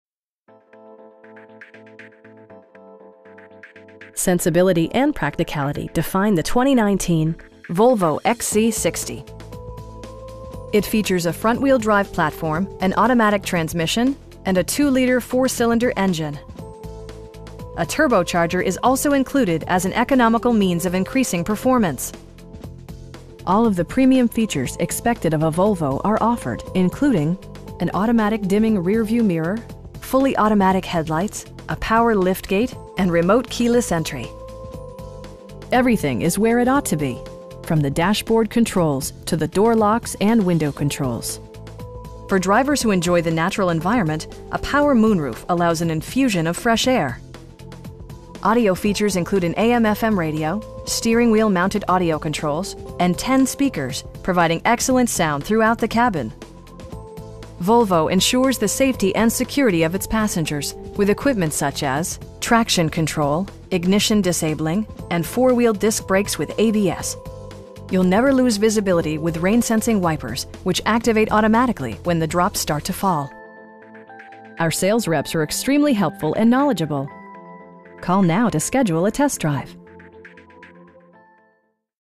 2019 Volvo XC60 T5 Momentum in Greensboro, NC 27407
Sensibility and practicality define the 2019 Volvo XC60. It features a front-wheel-drive platform, an automatic transmission, and a 2 liter 4 cylinder engine. A turbocharger is also included as an economical means of increasing performance. All of the premium features expected of a Volvo are offered, including: a power seat, a trip computer, an automatic dimming rear-view mirror, an outside temperature display, fully automatic headlights, a power liftgate, and remote keyless entry. Everything is where it ought to be, from the dashboard controls to the door locks and window controls. For drivers who enjoy the natural environment, a power moon roof allows an infusion of fresh air. Audio features include an AM/FM radio, steering wheel mounted audio controls, and 10 speakers, providing excellent sound throughout the cabin. Volvo ensures the safety and security of its passengers with equipment such as: head curtain airbags, front side impact airbags, traction control, brake assist, anti-whiplash front head restraints, ignition disabling, an emergency communication system, and 4 wheel disc brakes with ABS. You'll never lose visibility with rain sensing wipers, which activate automatically when the drops start to fall. Our sales reps are extremely helpful & knowledgeable. We'd be happy to answer any questions that you may have.